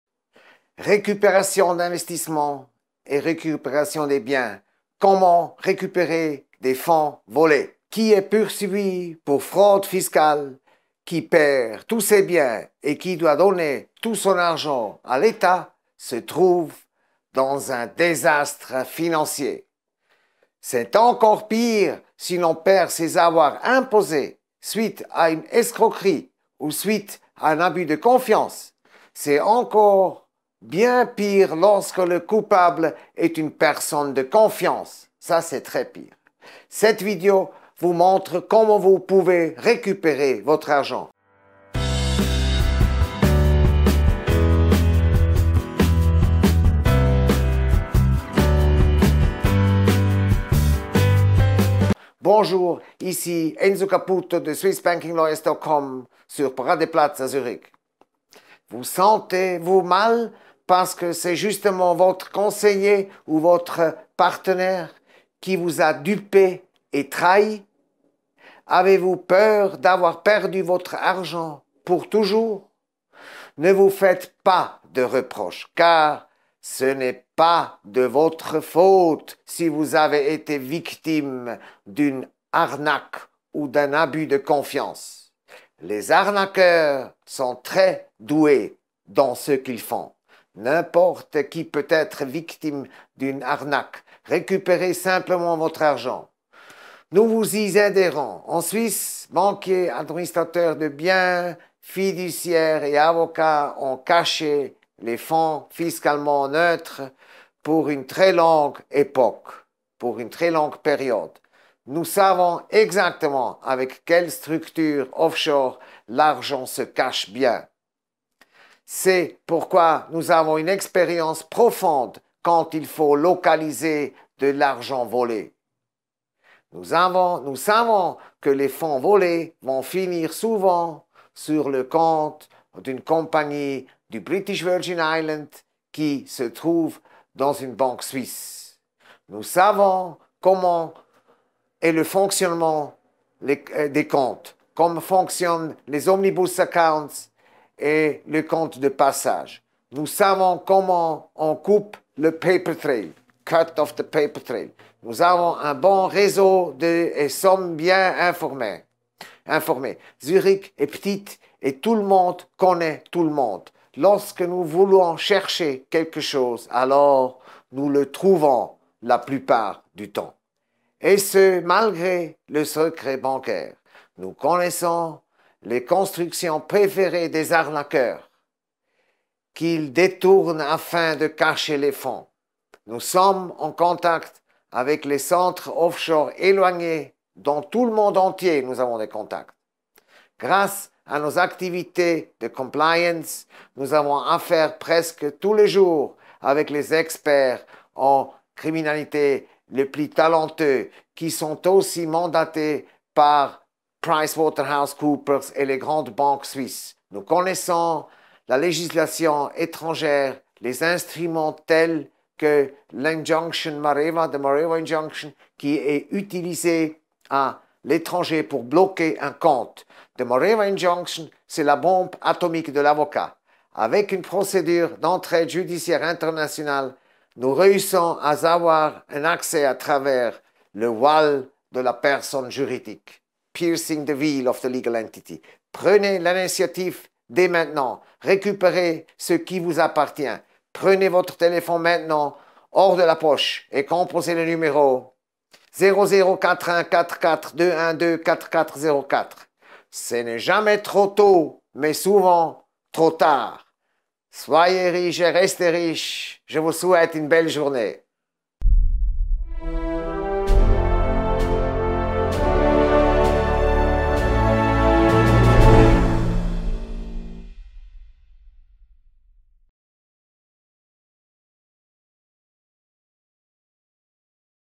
Récupération d‘investissement & de biens –Comment récupérer des fonds volés
Lorsque l‘on est poursuivi pour fraude fiscale, que l’on perd tous ses biens et que l’on doit de l’argent à l’Etat, c’est un désastre financier. C’est encore pire si l’on perd ses avoirs imposés suite à une escroquerie ou à un abus de confiance. C’est encore bien pire lorsque le coupable est une personne de confiance. Cette vidéo vous montre comment vous pouvez récupérer votre bien. ...

⇨ Multilingual service
⇨ 24/7 support